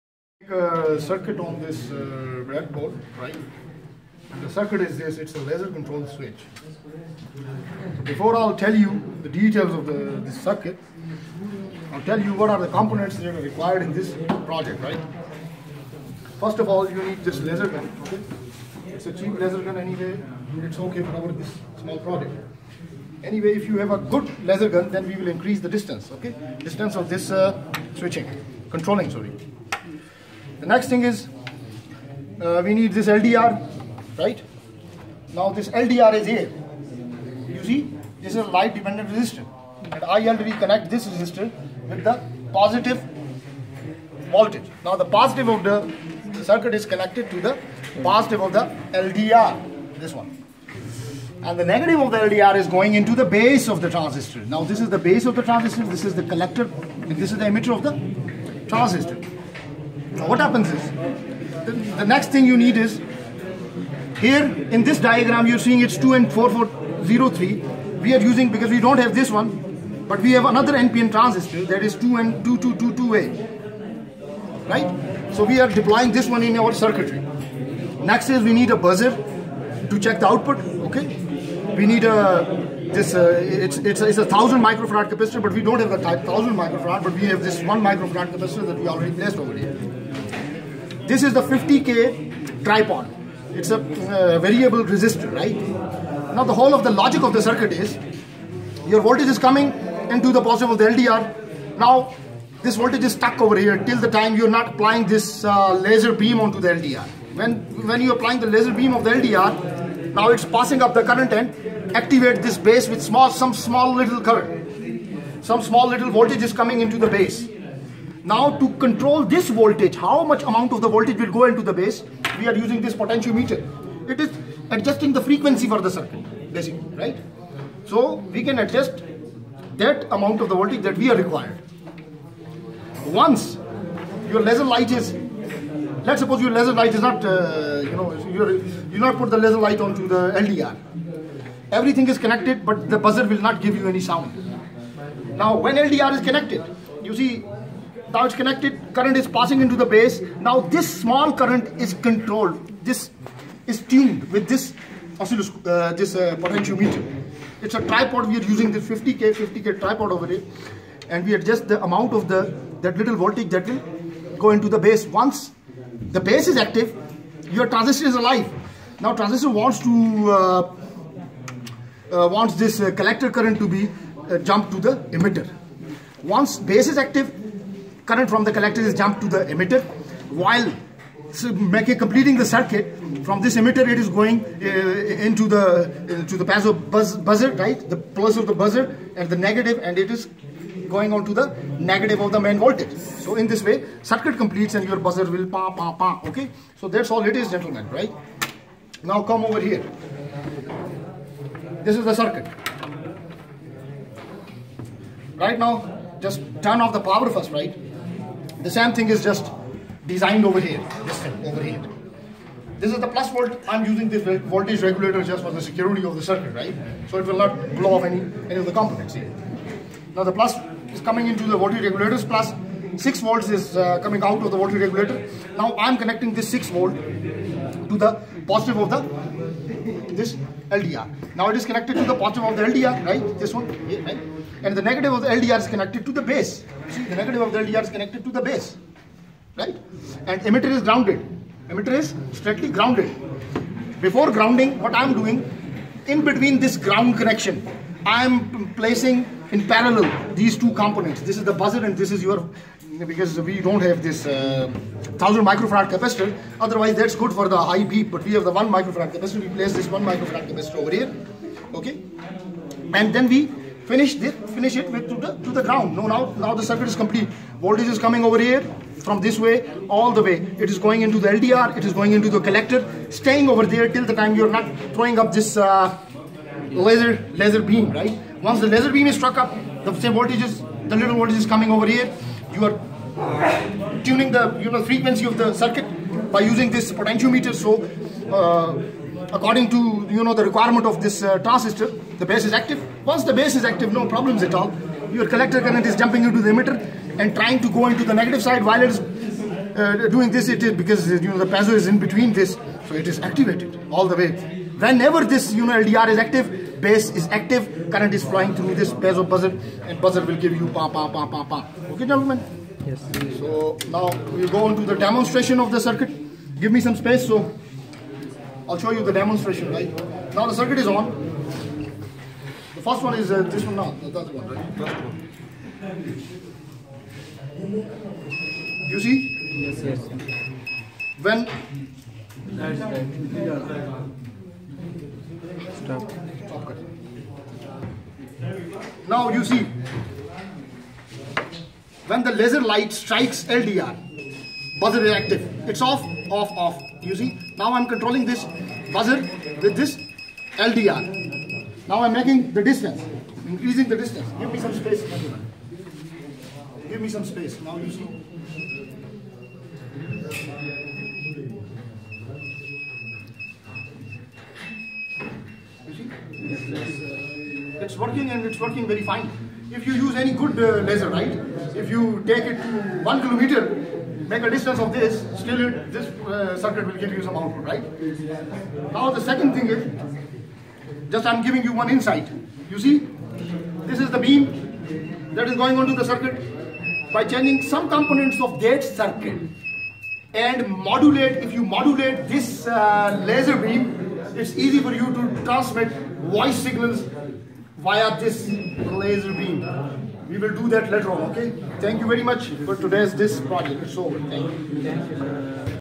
Laser Switch
A good video to understand the use of lasers and how to control electronic circuits from a distance with the help of laser beam.